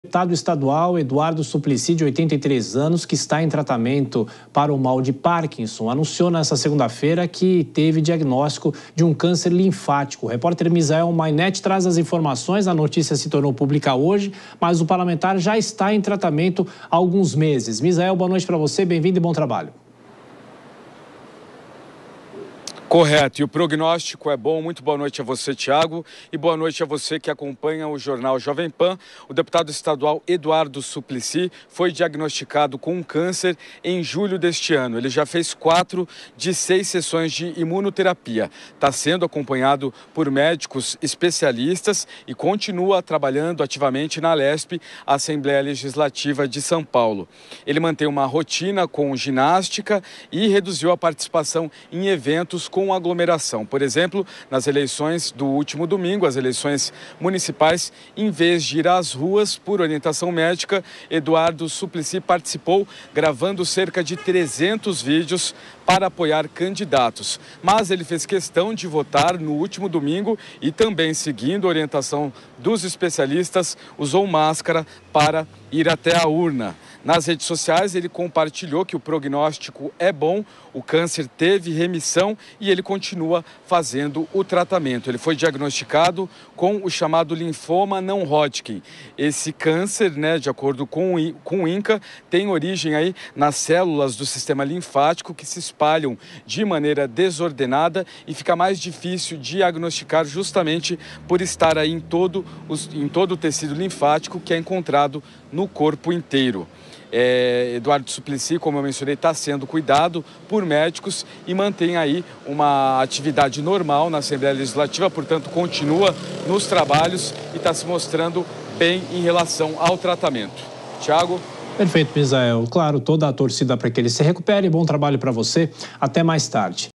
Eduardo Suplicy anuncia que está com câncer linfático
O deputado estadual Eduardo Suplicy (PT), de 83 anos, revelou que foi diagnosticado com câncer linfático. A notícia surge enquanto ele segue em tratamento contra o mal de Parkinson, diagnosticado em julho deste ano.